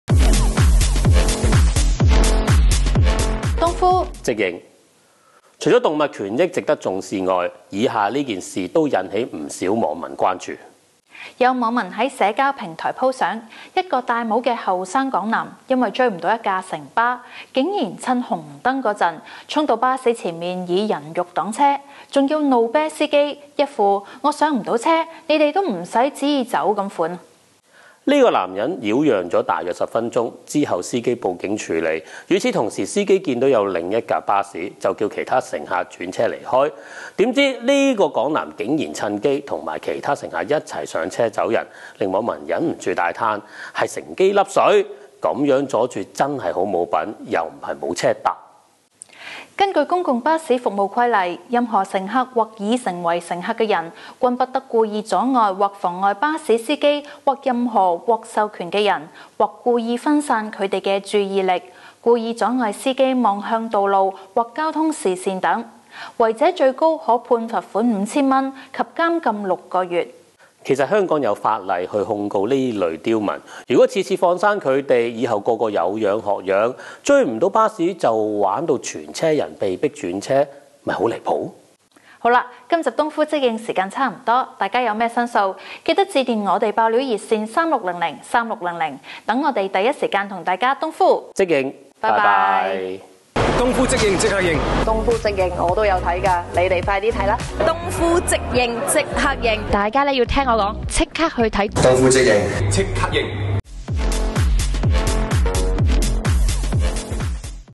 【on.cc東網】東呼即應：港男送車尾發爛渣 人肉擋巴士逼停車
等下一班車真係咁難？

【東呼即應：港男送車尾發爛渣　人肉擋巴士逼停車】